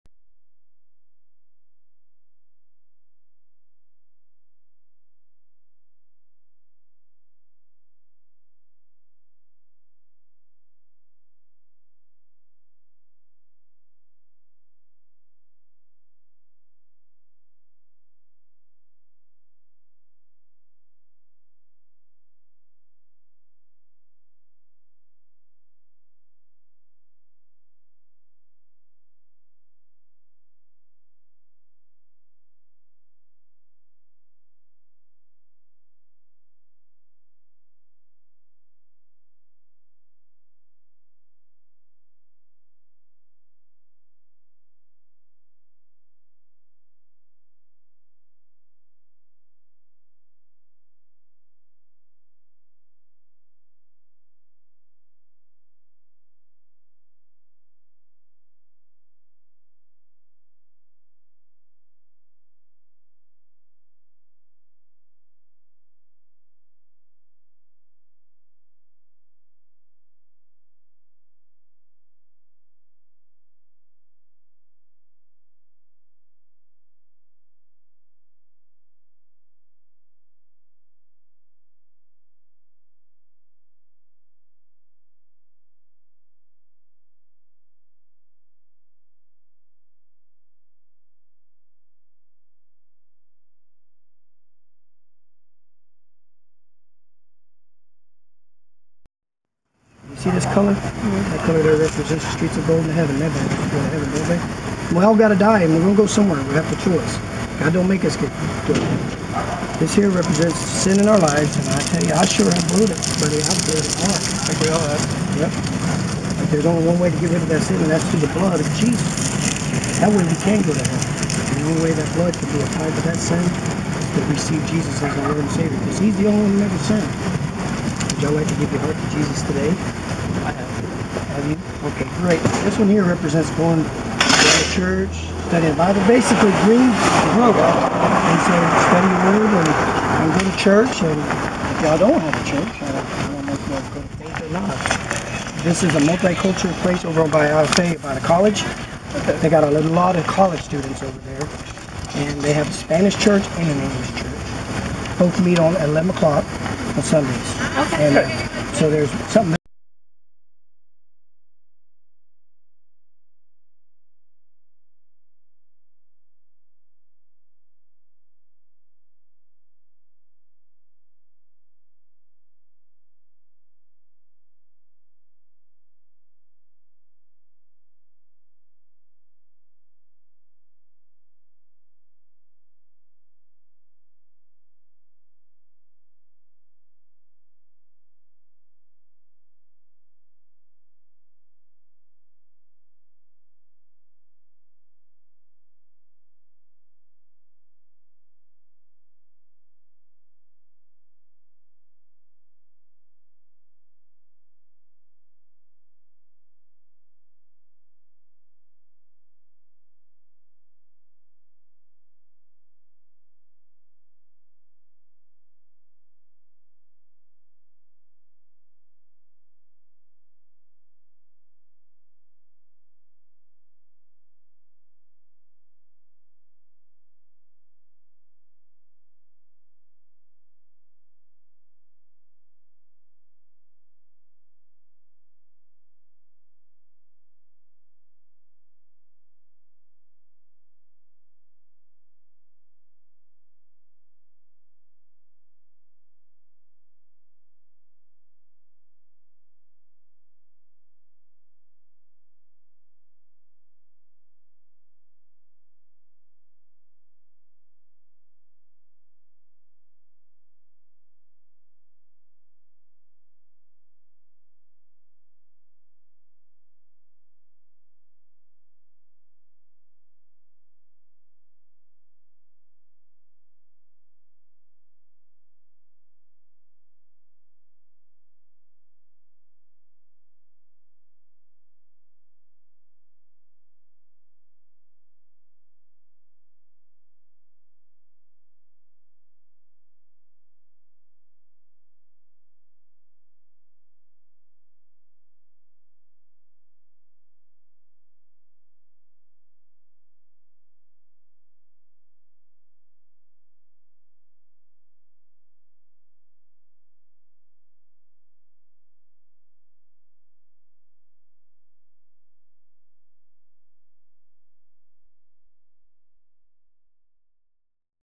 Community outreach 2013
Flowing Fountain Ministries reaching out to the community right in front of our home